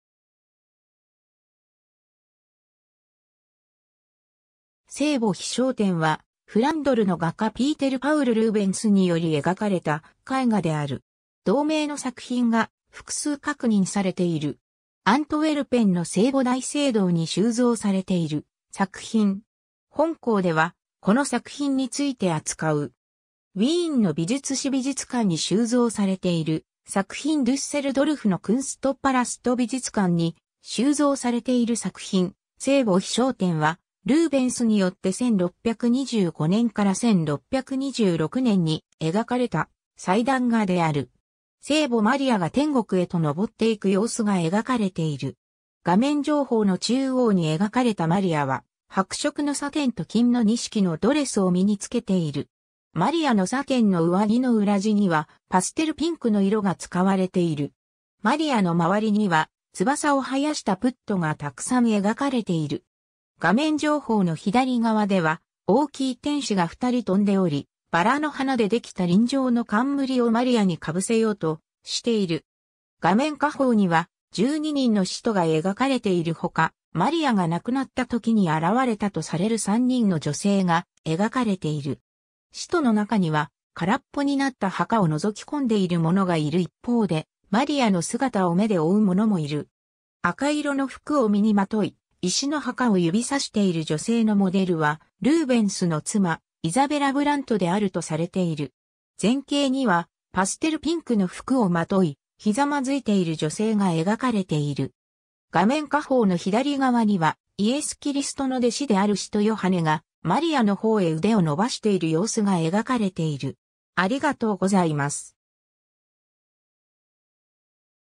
聖母被昇天 (ルーベンス)
ピーテル・パウル・ルーベンスの作品
『聖母被昇天』（せいぼひしょうてん）は、フランドルの画家ピーテル・パウル・ルーベンスにより描かれた絵画である。
同名の作品が複数確認されている。
 アントウェルペンの聖母大聖堂に収蔵されている作品。
本項では、この作品について扱う。
 ウィーンの美術史美術館に収蔵されている作品 デュッセルドルフのクンストパラスト美術館に収蔵されている作品 『聖母被昇天』（蘭: Tenhemelopneming van Maria, 英: Assumption of the Virgin Mary）は、ルーベンスによって1625年から1626年に描かれた祭壇画である。
 聖母マリアが天国へと昇っていく様子が描かれている。
画面上方の中央に描かれたマリアは、白色のサテンと金の錦のドレスを身につけている。
マリアのサテンの上着の裏地には、パステルピンクの色が使われている。
 マリアの周りには、翼を生やしたプット（幼児）がたくさん描かれている。
画面上方の左側では、大きい天使が2人飛んでおり、バラの花でできた輪状の冠をマリアに被せようとしている。
 画面下方には、12人の使徒が描かれている他、マリアが亡くなったときに現れたとされる3人の女性が描かれている。
使徒の中には、空っぽになった墓を覗き込んでいる者がいる一方で、マリアの姿を目で追う者もいる。
 赤色の服を身にまとい、石の墓を指さしている女性のモデルは、ルーベンスの妻イザベラ・ブラントであるとされている。
前景には、パステルピンクの服をまとい、ひざまずいている女性が描かれている。
 画面下方の左側には、イエス・キリストの弟子である使徒ヨハネが、マリアの方へ腕を伸ばしている様子が描かれている。